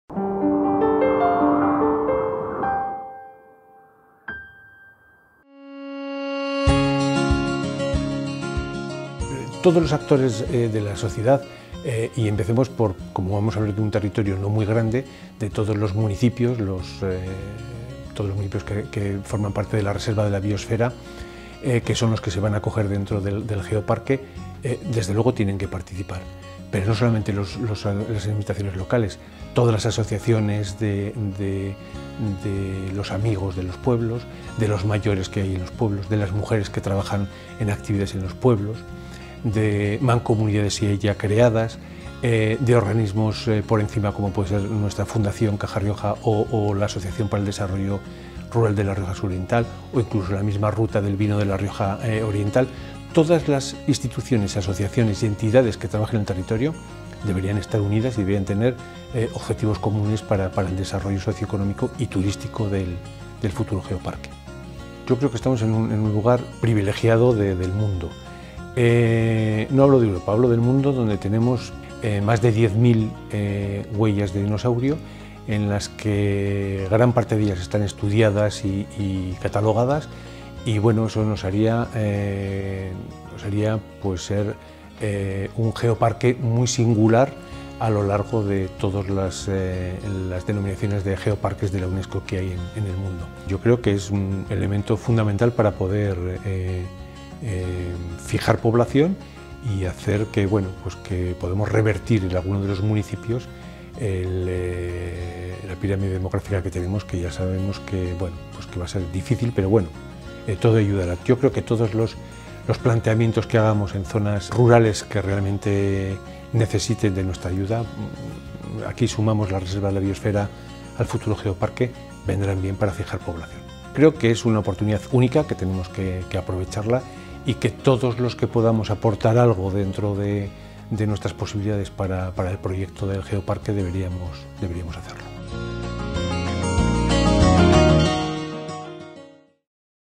PROYECTO GEOPARQUE LA RIOJA. Arturo Colina, director de Fundación Caja Rioja
🧩"Las instituciones, asociaciones y entidades que trabajen en el territorio deberían estar unidas y tener objetivos comunes para el desarrollo del proyecto de Geoparque de La Rioja"